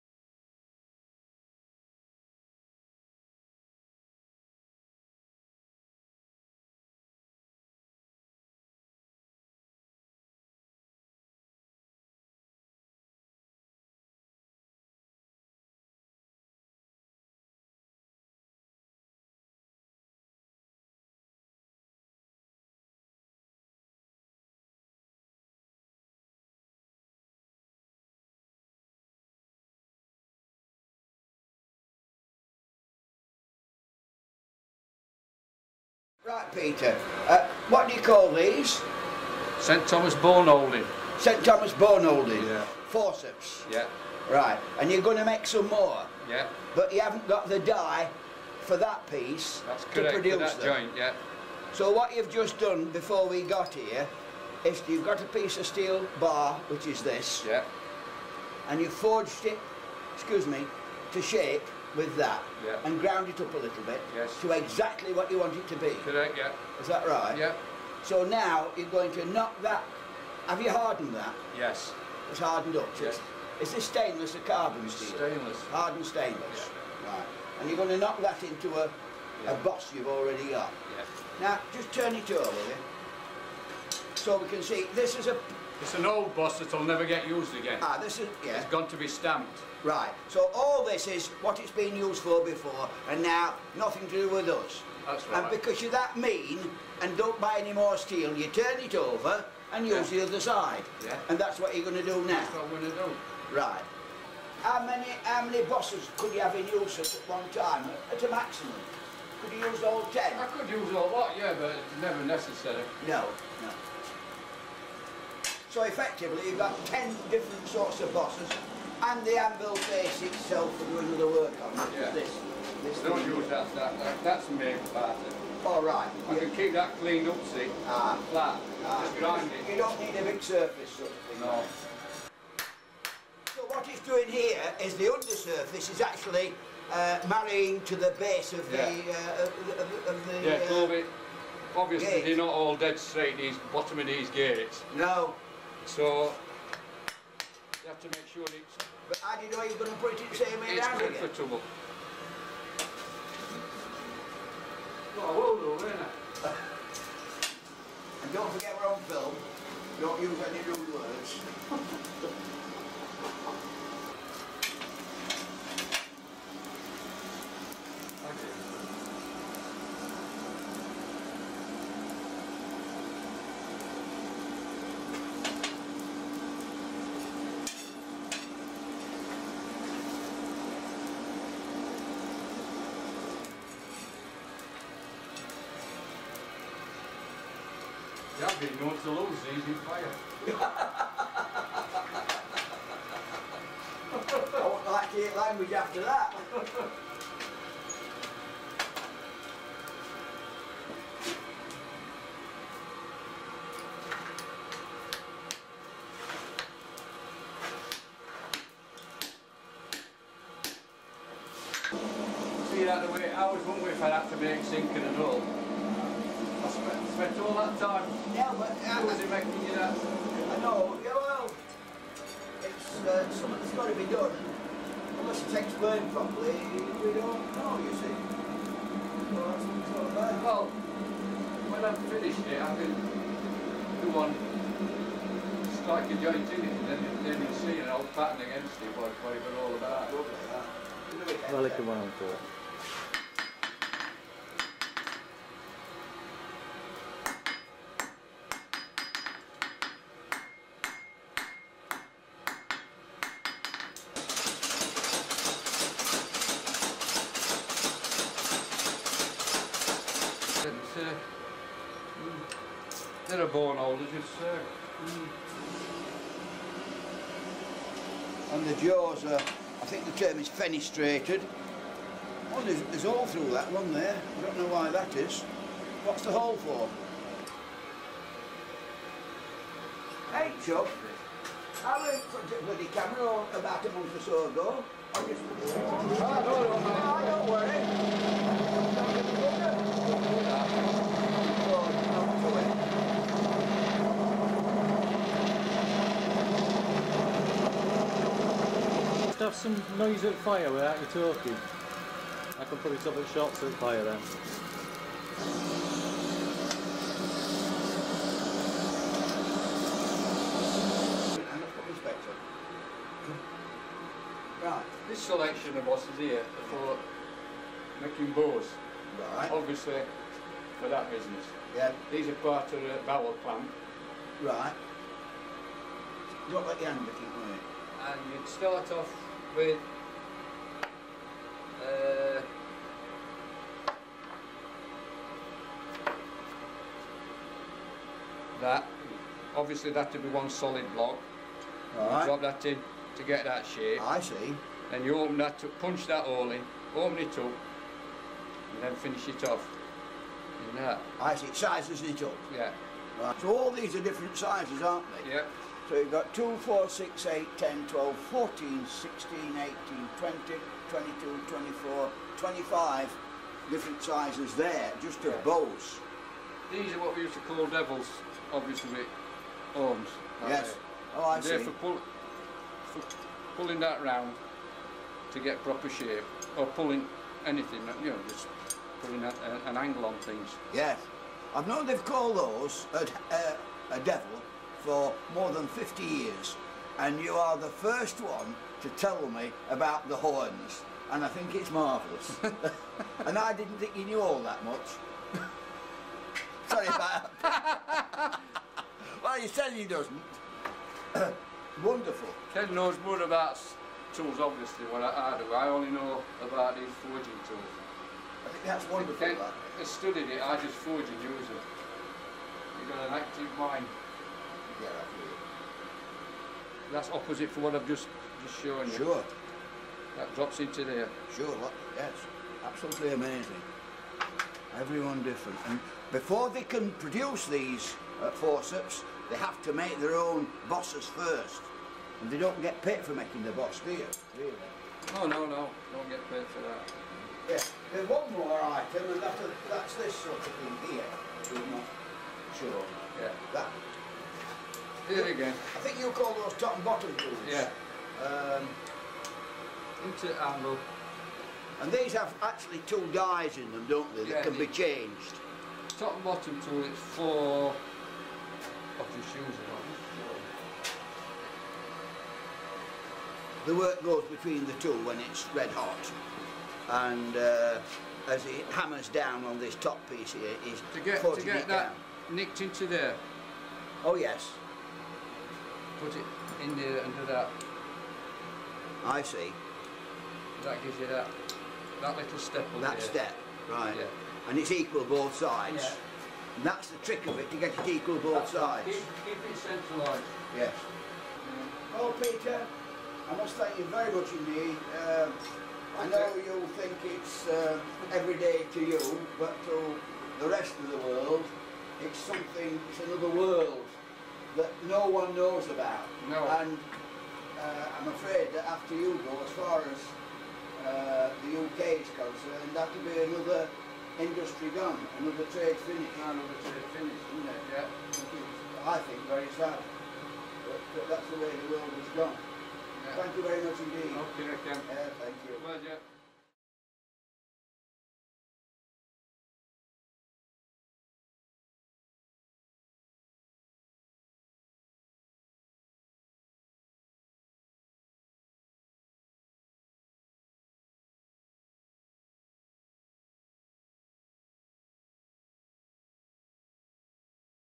Hand Forging Surgical Instruments - Pete Goss (2006)
Pete Goss continues to work at his forge down at Kelham Island Museum.   This film shows him practically demonstrating his skill as a hand forger to Ken Hawley whose enthusiasm for explaining the processes involved shines through.  Pete's particular specialism is hand forging precision surgical instruments.

The film adds to a series filmed in the mid-1960s which Ken Hawley inspired.  Ken had a continuing concern that he was witnessing the disappearance of a wide range of craft skills on which Sheffield's reputation had been built over the two previous centuries.

To find out more about the Hawley Collection and to donate, visit our